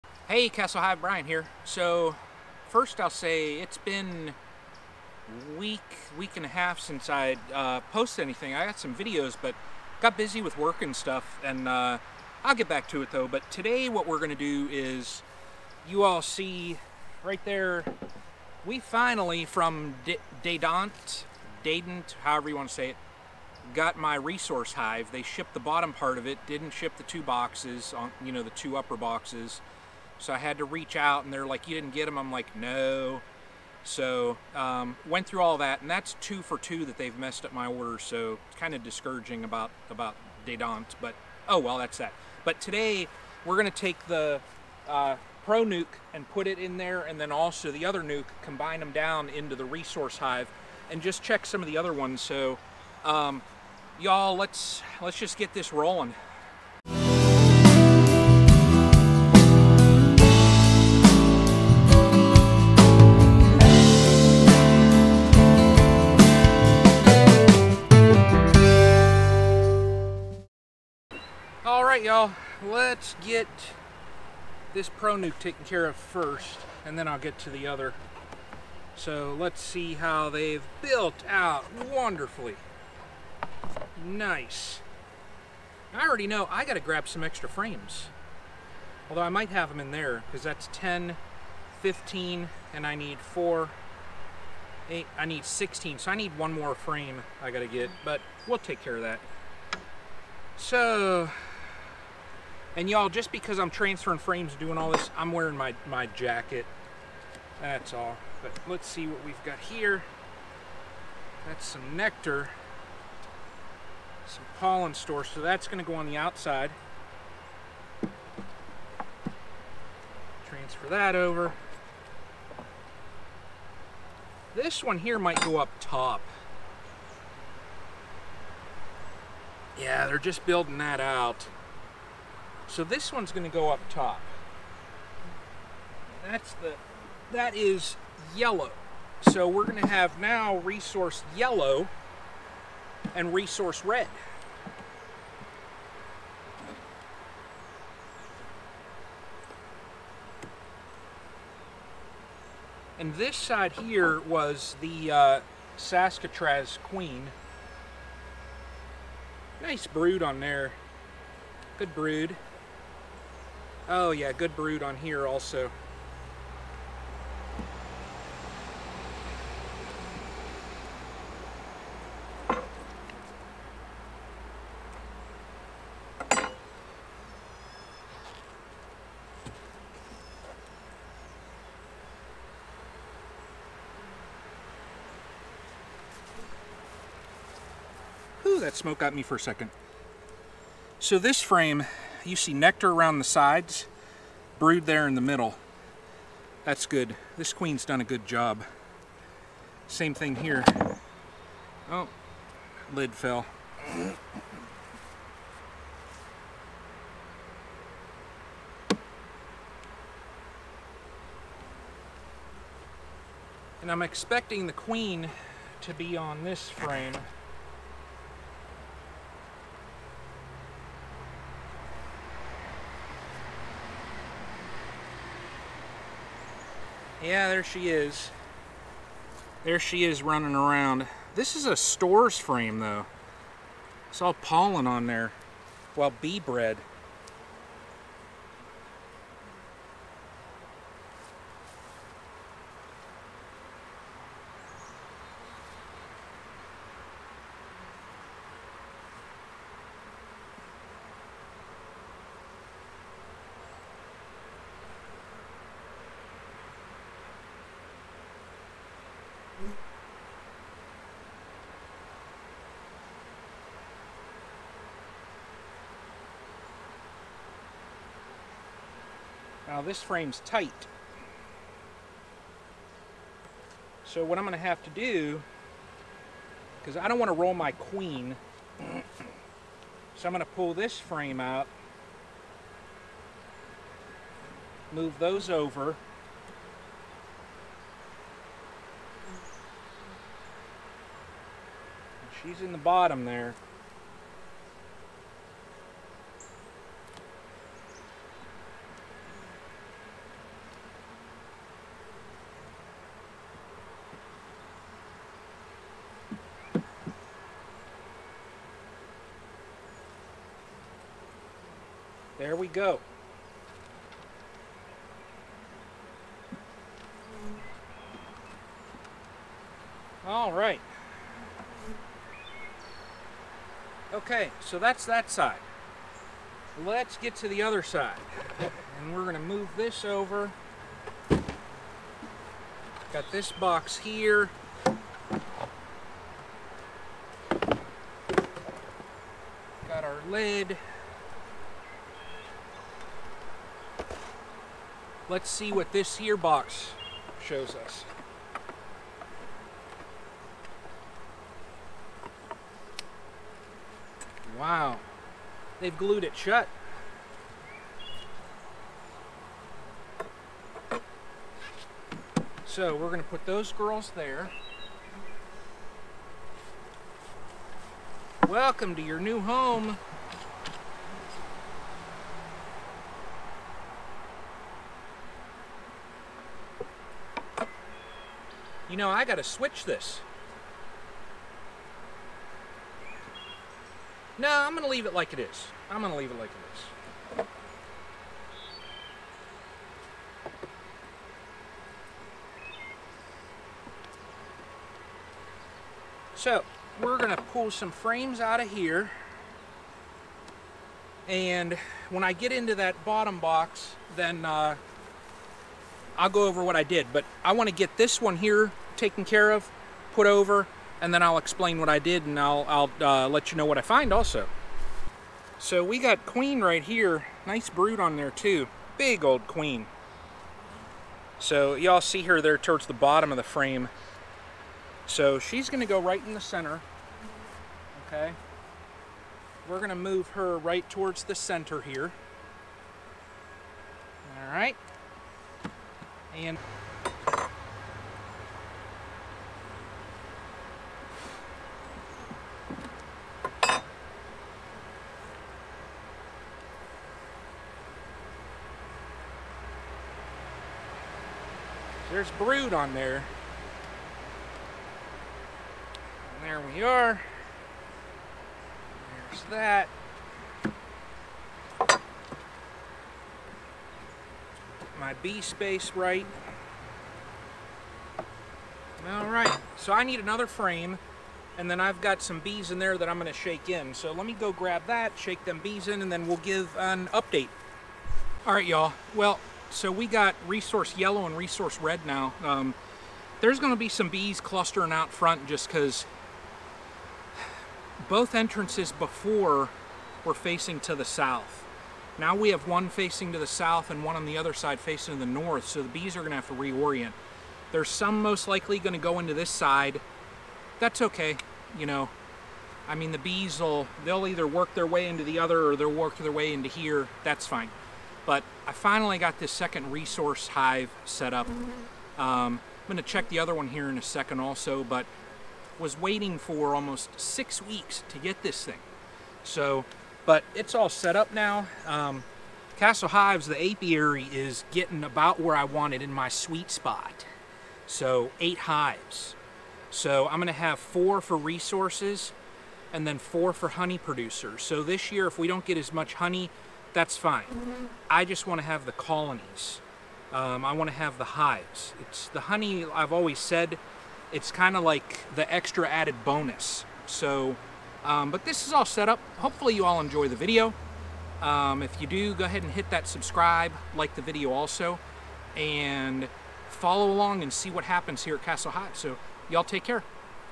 Setting up a Resource Hive - July 6th
Well all,

It finally arrived. Took Dadant near 6 weeks to get me my entire order, but I was finally able to setup the 2nd Resource Hive, AKA Double NUC. Castle Hives will now have Resource Yellow and Resource Red, in addition to the other. Both Red and Yellow will have Saskatraz Bees.

Brian